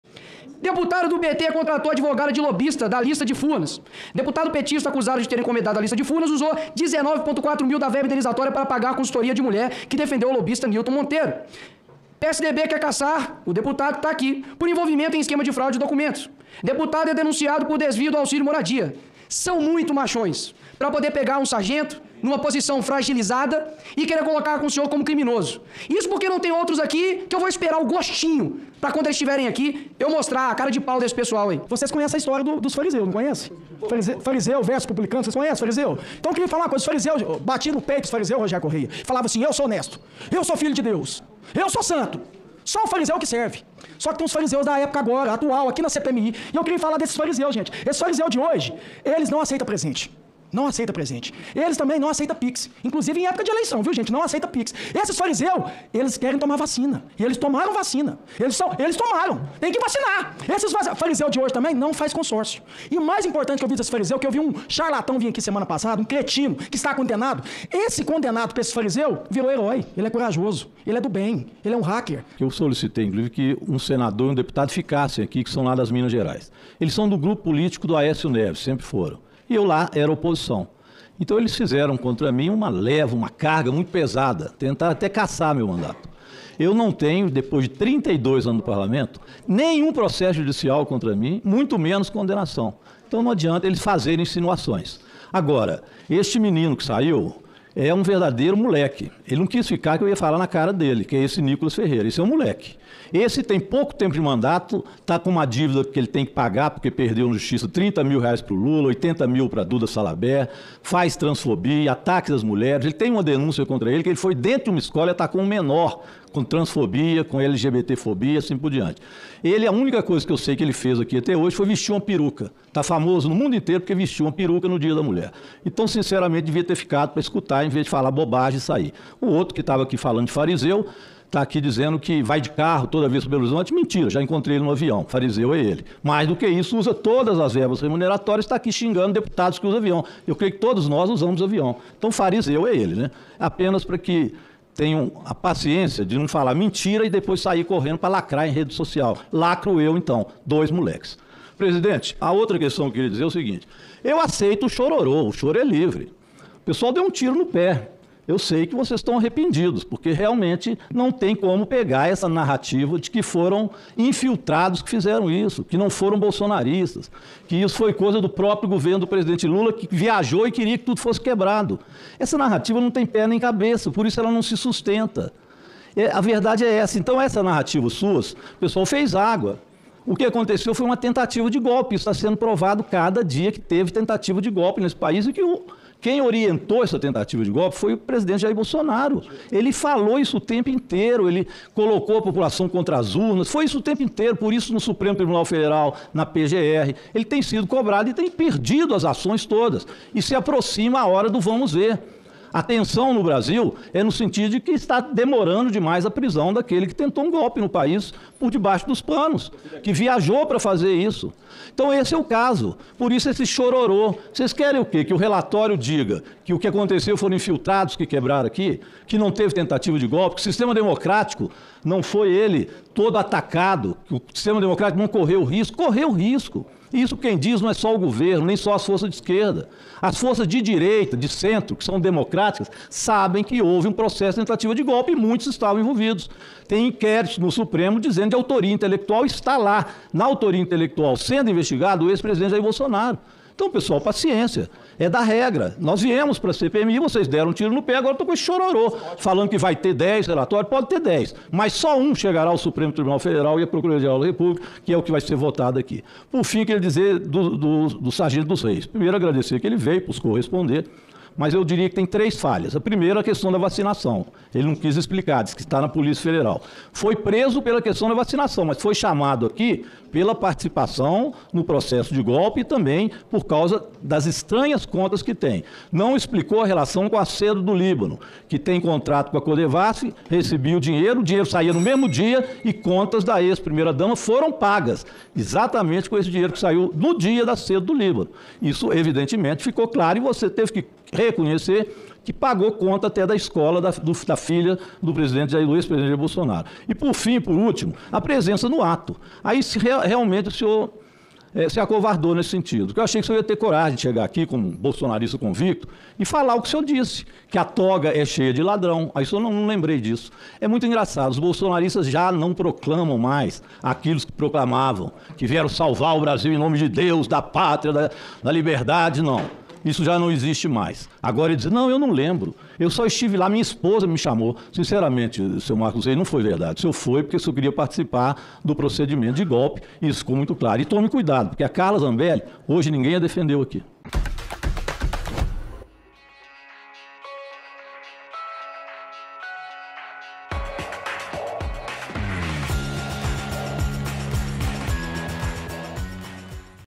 "O CHORO É LIVRE", DIZ ROGÉRIO CORREIA SOBRE A TURMA DA LACRAÇÃO BOLSONARISTA NA CPMI | Cortes 247
Confira trechos da sessão da CPMI do 8 de Janeiro que ouviu o sargento Luis Marcos dos Reis, que integrava a equipe de Jair Bolsonaro na presidência. Extremistas do bolsonarismo em Minas Gerais, o deputado federal Nikolas Ferreira (PL-MG) e o senador Cleitinho (Republicanos-MG) atacaram o petista Rogério Correia e depois não ficaram para ouvir a resposta. O deputado do PT chamou Nikolas de moleque.